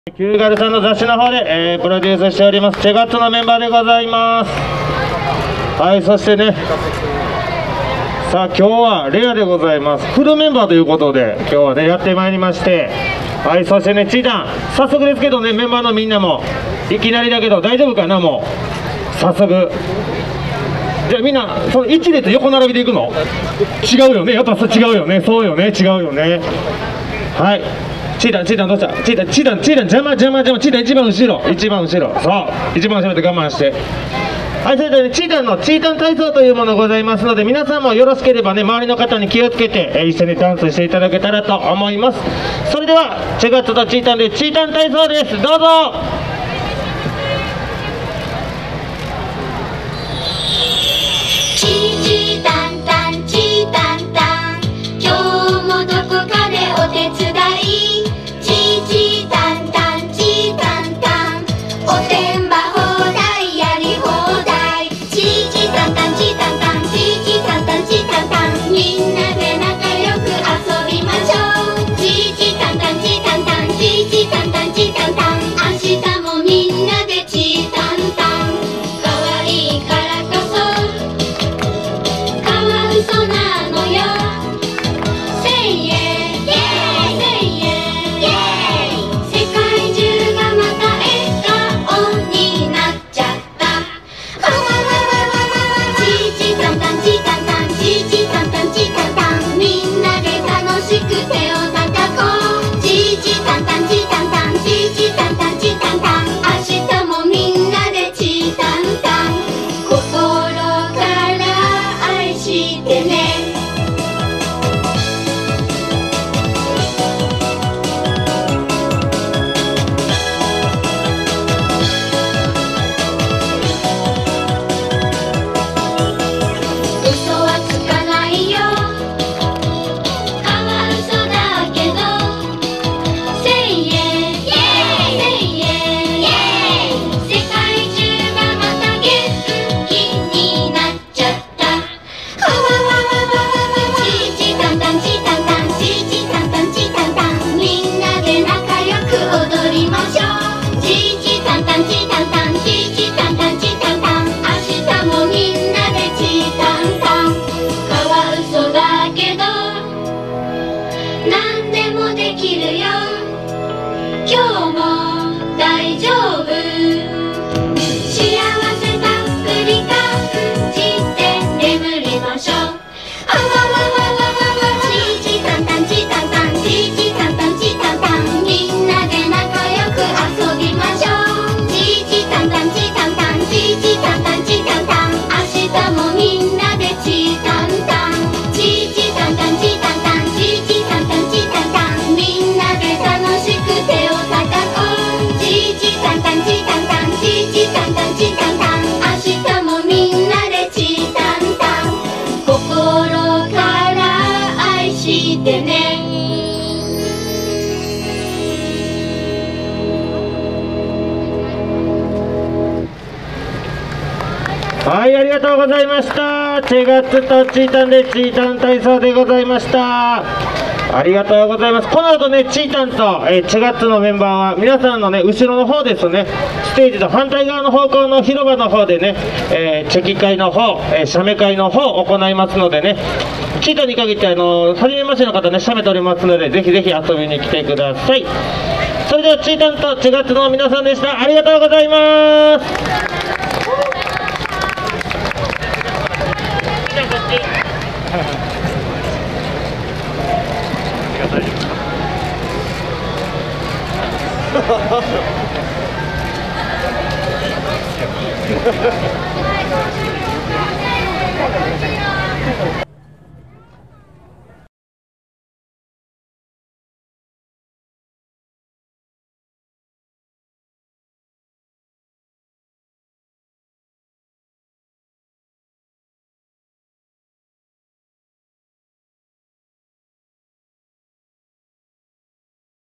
ちぇガッツ！ [R5C/4K60p]（代々木公園ケヤキ並木 2023.11.25）
【ちぇガッツ！ ライブ情報】
11月25日(土)
『カンボジア水祭り』
場所:都立代々木公園ケヤキ並木

① 13時30分〜　ステージ

ちぇガッツ！
アイドルユニット：ちぇガッツ！です。
ちぃたん☆
 と仲間たちで活動しています☆

EOS R5C
RF70-200mmF4.0 L
C-LOG3　撮影

japanese idol girls group